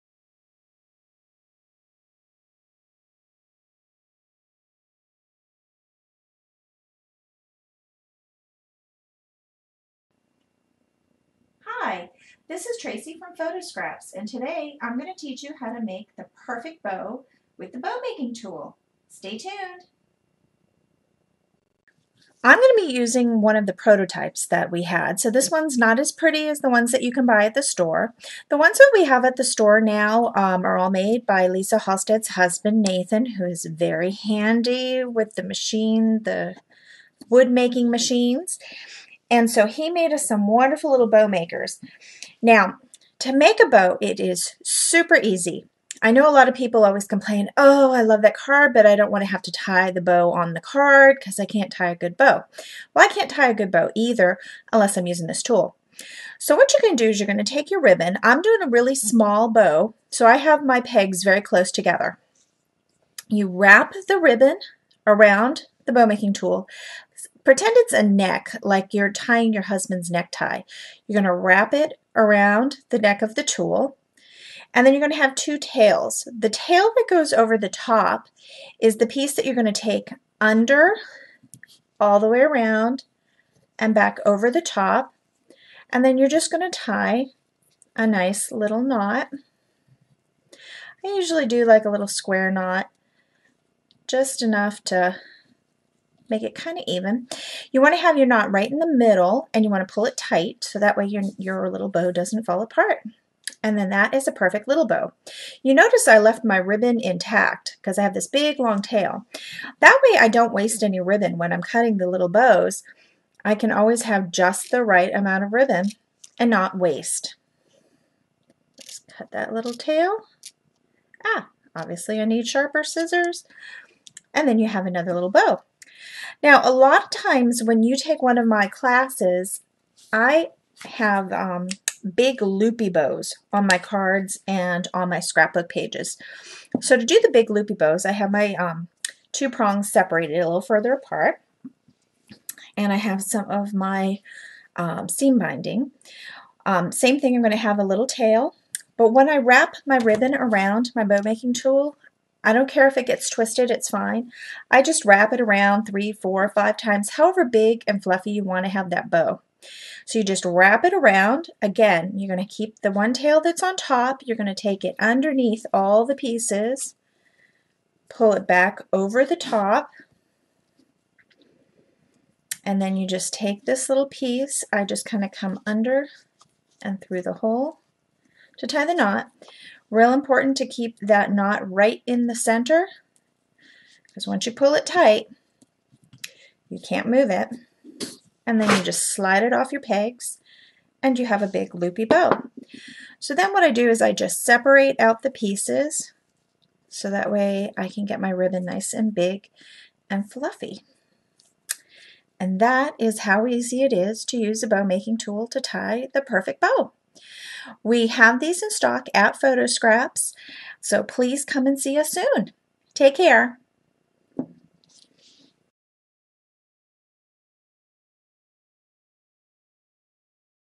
Bow Tying Made Easy
Tracy show you how to tie an easy bow with the Bow Making Tool. Tools
available at Photo Scraps Store.